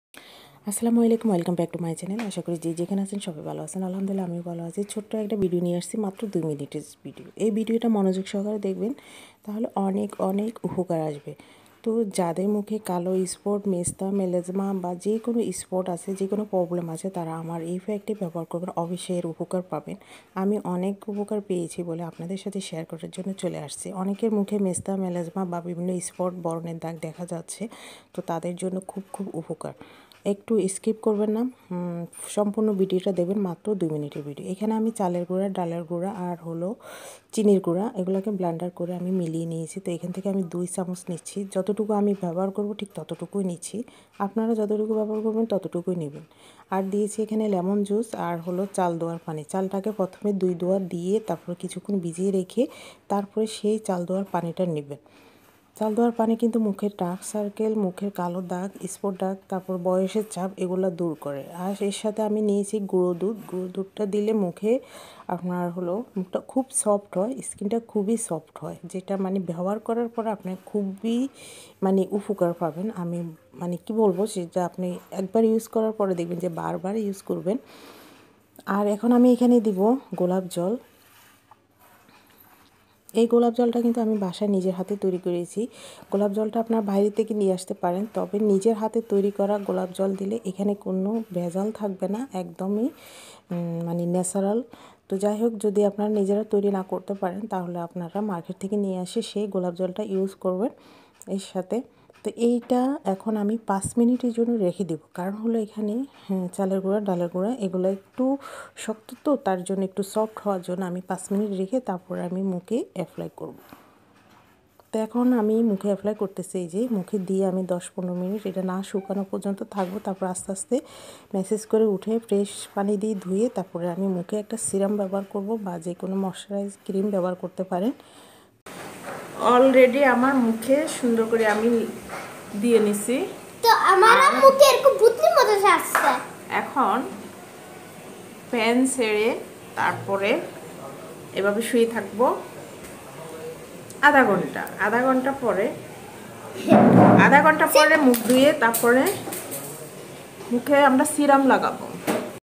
সাত দিনের ভিতরে মুখের মেছতা মেলেজমা কালো স্পট দূর করার জাদুকরি ফ্যাক।
মেস্তা, কালো স্পট দূর করার জাদুকরি ফ্যাক